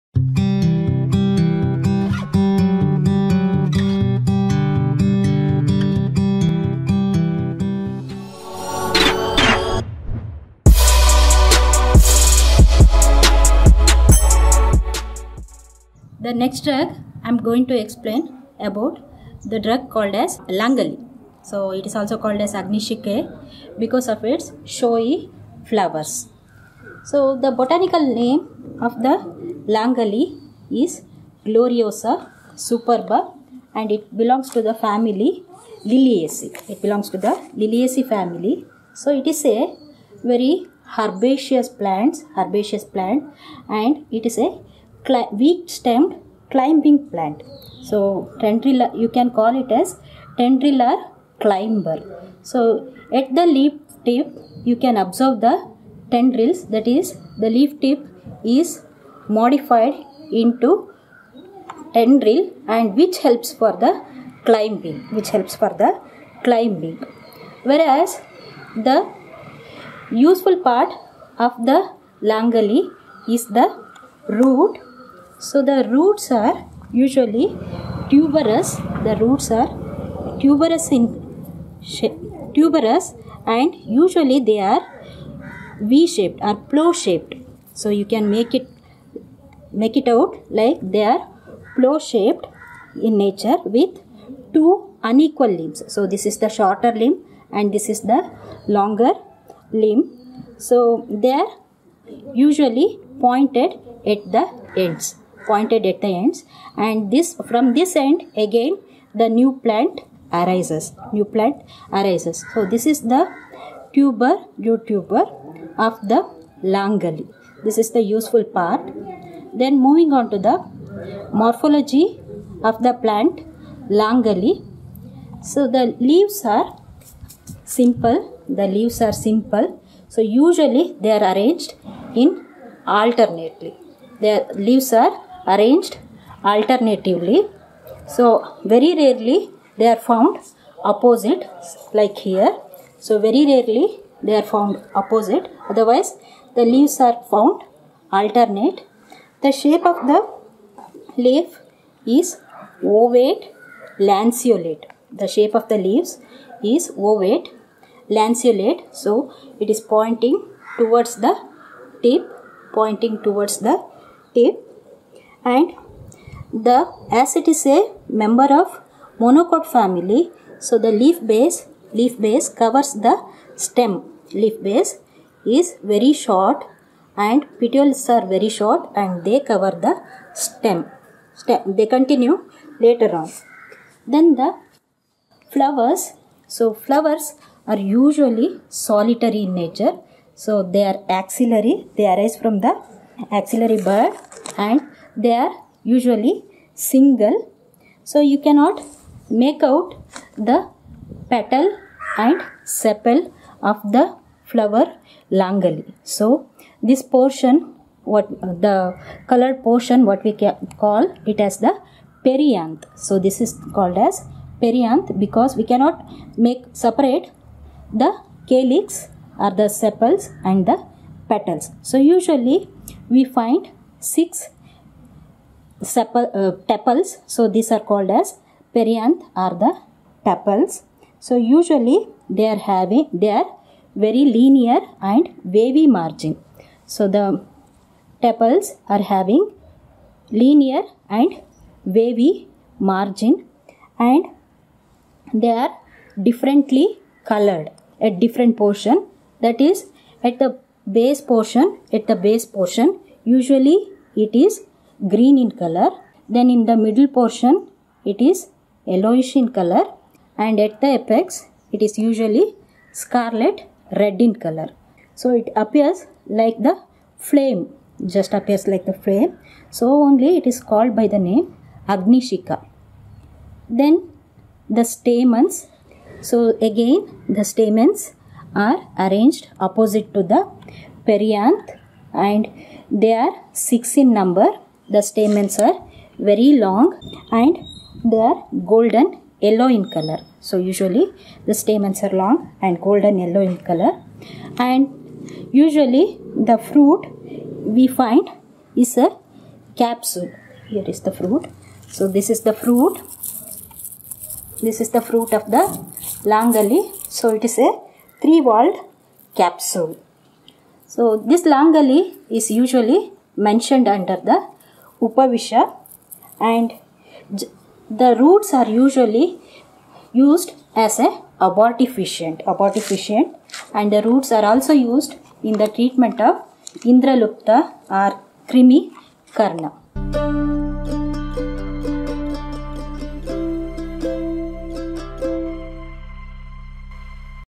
Medicinal Plants Documentary: LANGALI (Glosiosa superba L.)
Medicinal plants documentary: LANGALI (Glosiosa superba L.) narrated by Dr. Shilpa KN, BAMS, MD(Ayu).

The video recorded at Aln Rao Memorial Ayurvedic Medical College, Koppa.